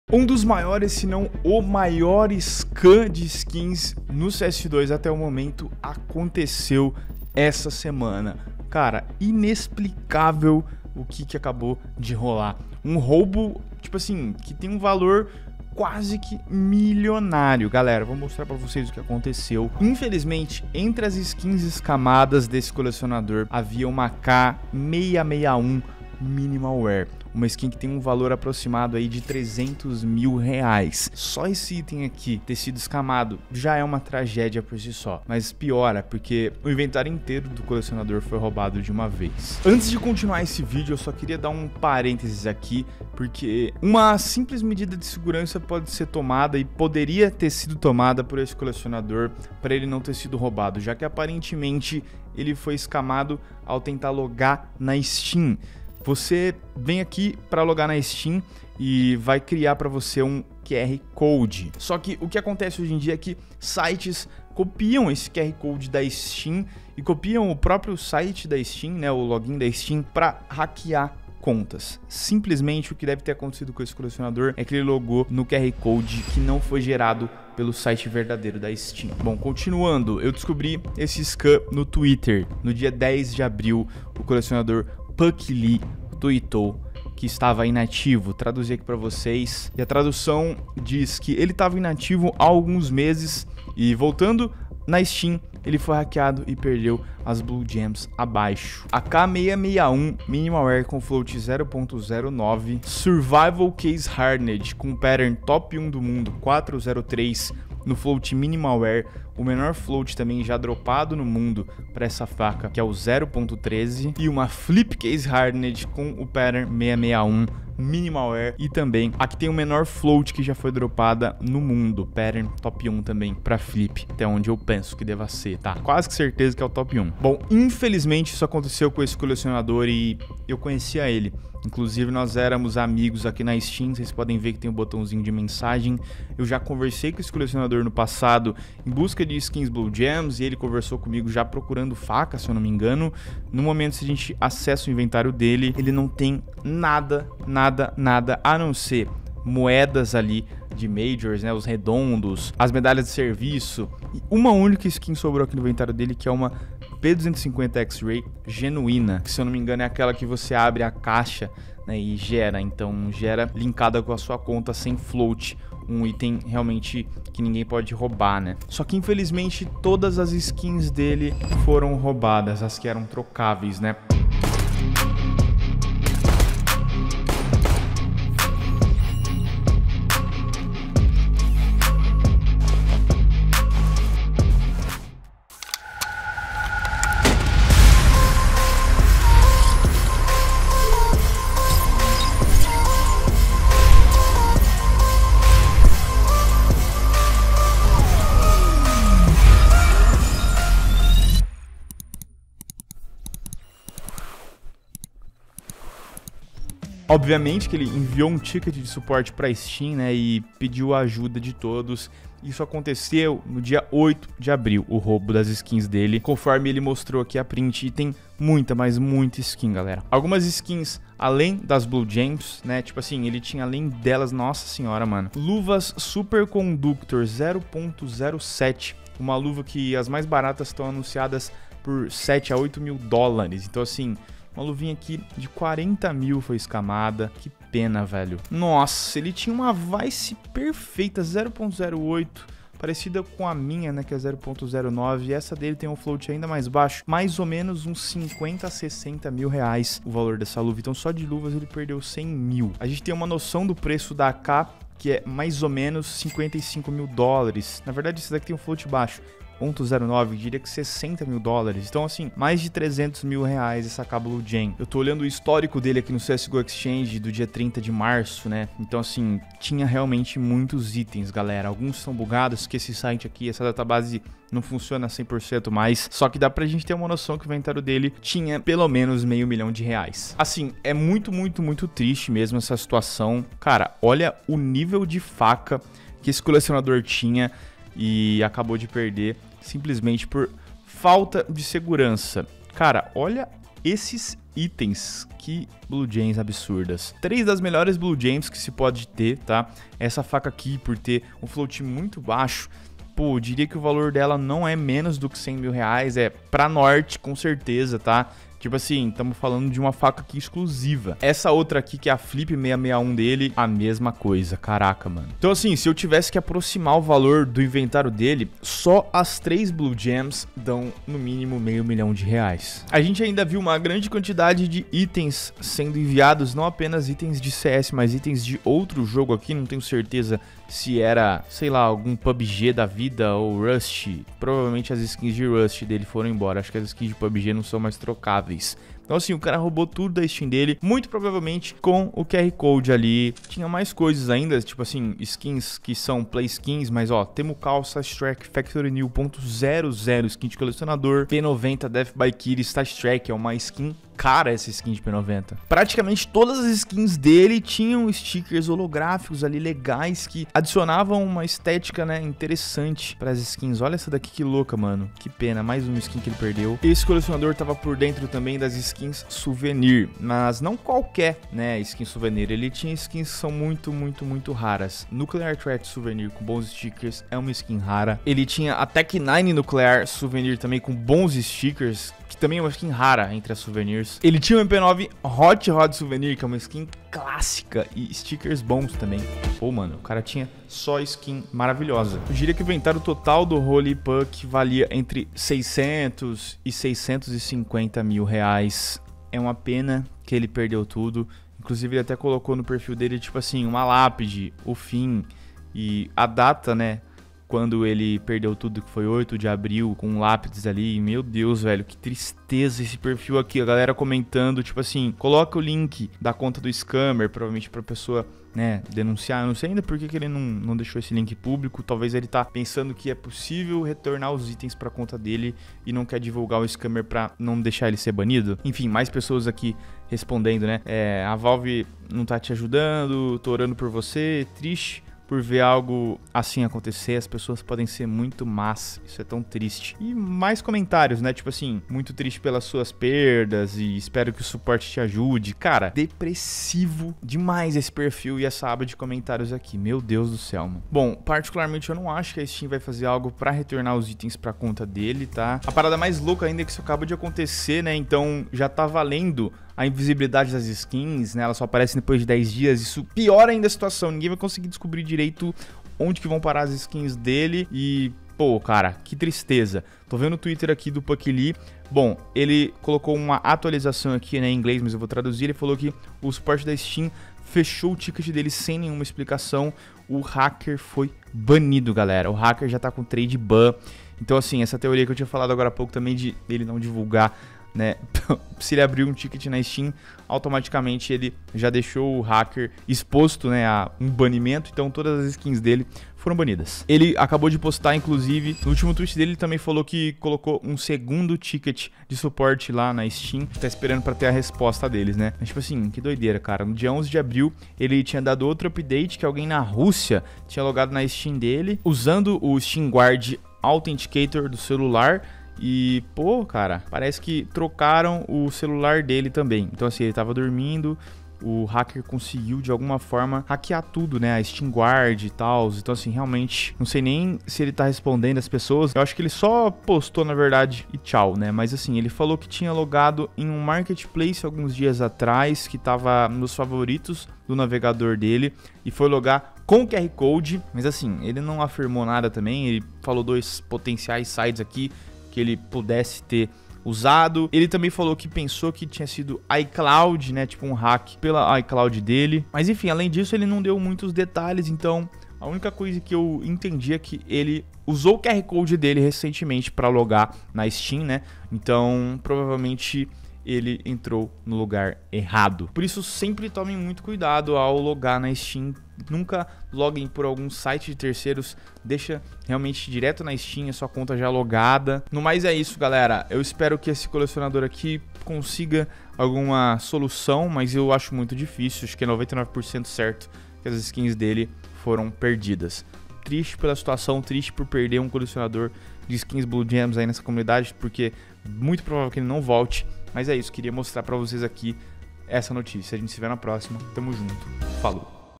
CUIDADO! Suas Skins Podem Estar Correndo Risco... (O Maior Scam do CS 2)
3 Skins Blue Gems foram scammadas essa semana no CS2 junto com diversas outras skins de um inventário de colecionador.